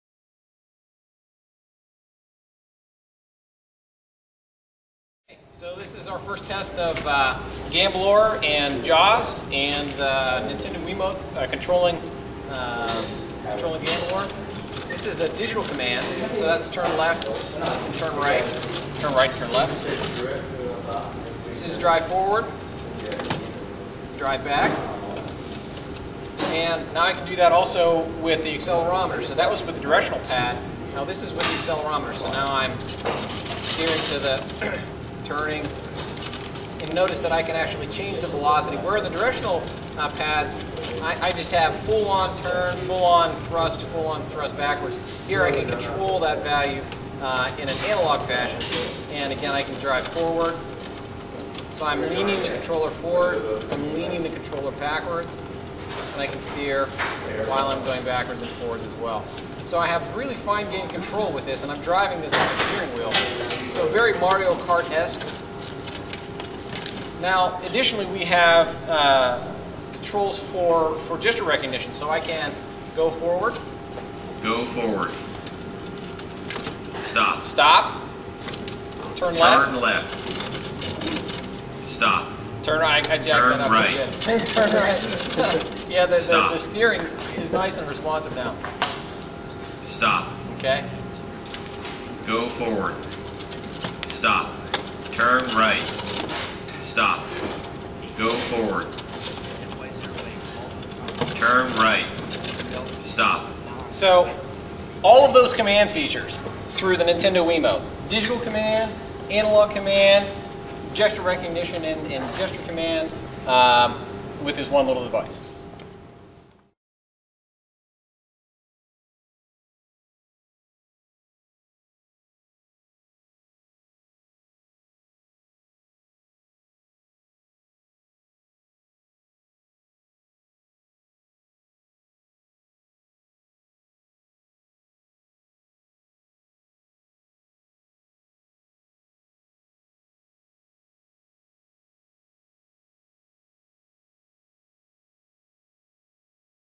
Robotic Gestural Control with JAUS and the Nintendo Wiimote
Part 2 of 2 videos demonstrating an unmanned control interface using JAUS, Gesture Recognition, and the Nintendo Wiimote.